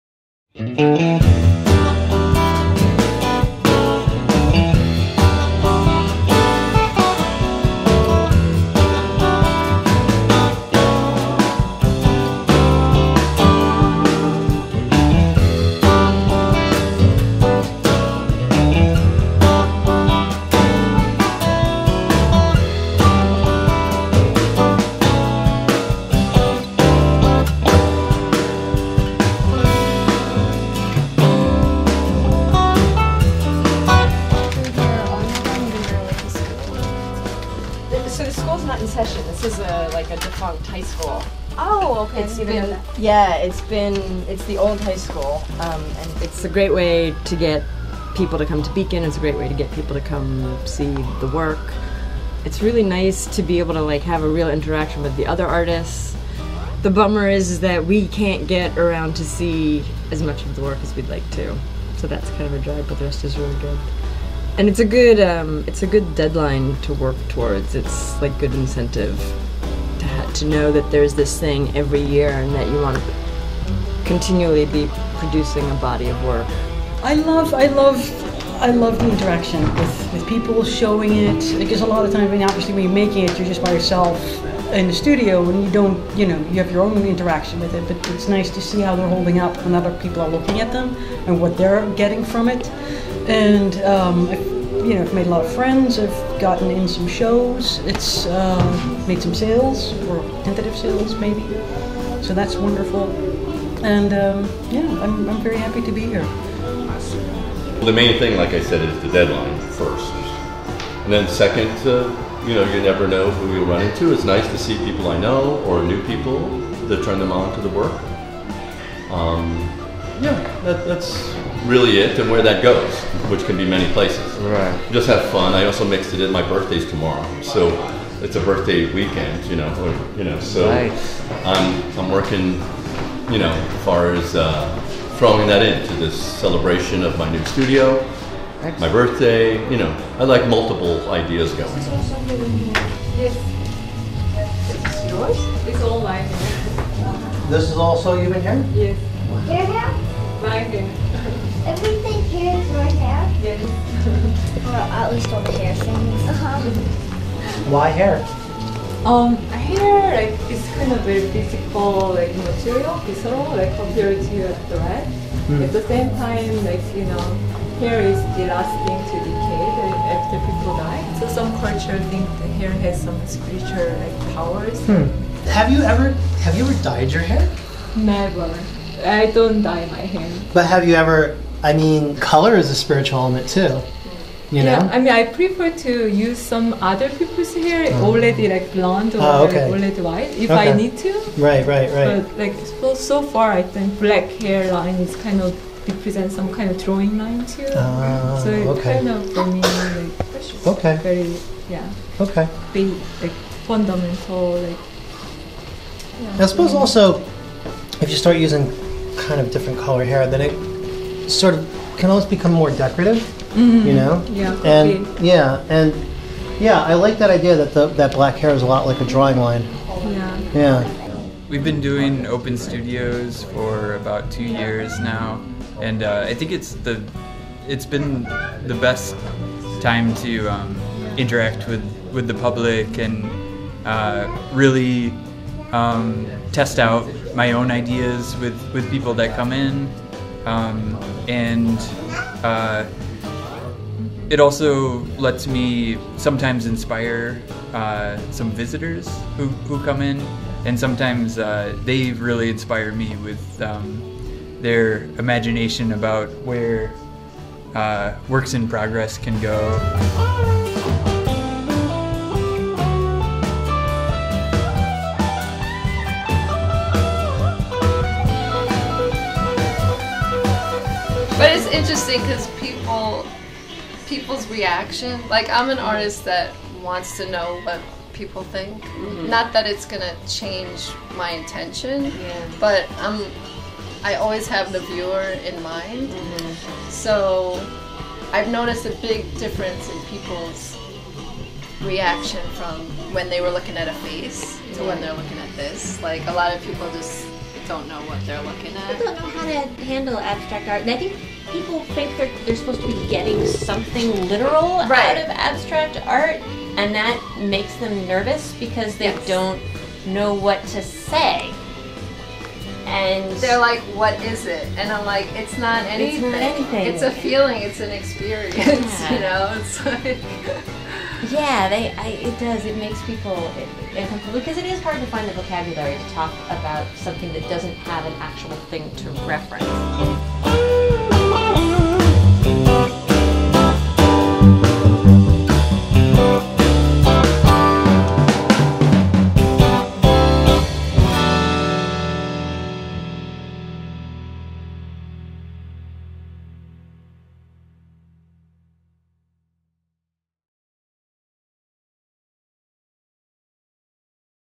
Beacon Open Studios 2016 The KUBE
Beacon Open Studios 2016 at KuBe
Beacon Open Studios 2016. I volunteered to create 4 short videos featuring as many artists I was able to visit in the two days.The videos are about the art and what open studios means for both the visitors and the artists. It turned out that a lot of Artists talked about not being able to visit other studios so this helps with that. We had 65 Artists participating, I couldn't get them all, just a small fraction.